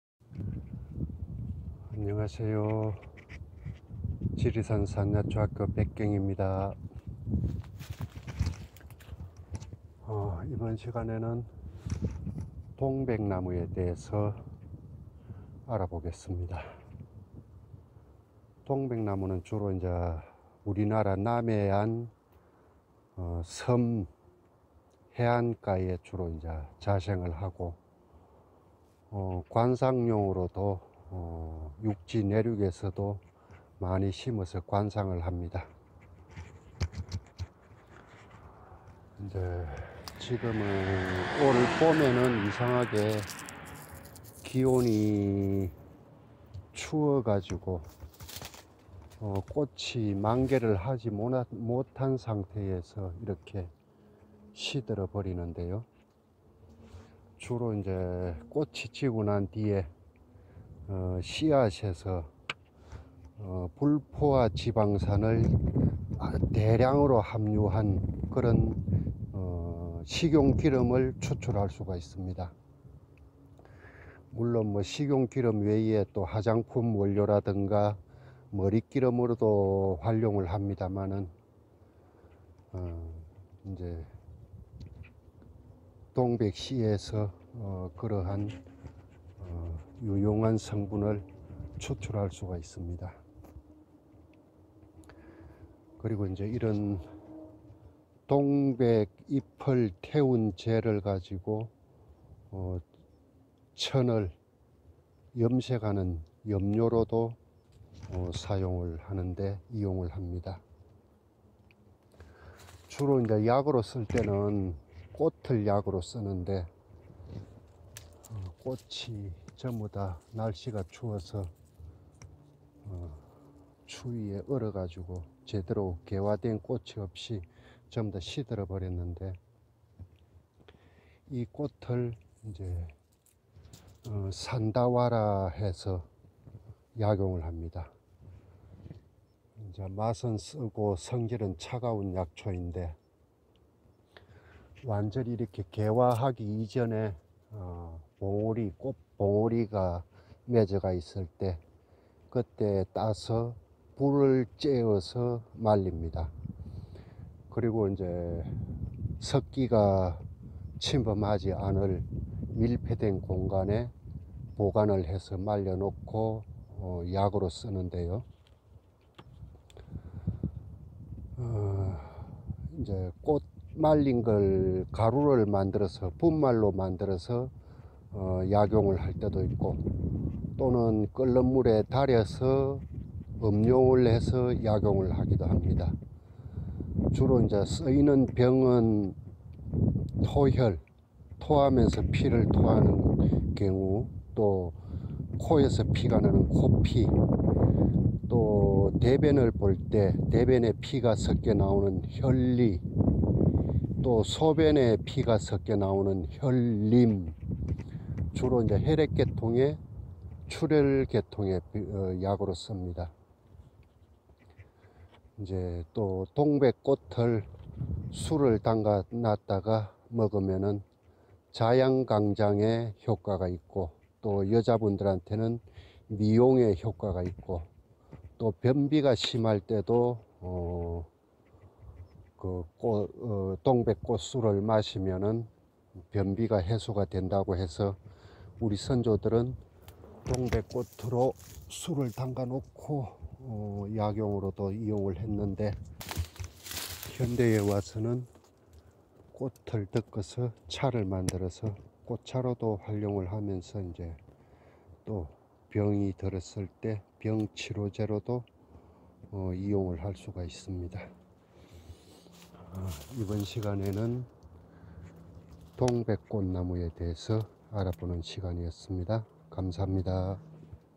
동백나무
토혈, 코피, 혈리, 혈림, 화장품, 머릿기름, 식용기름등 약용하는 동백나무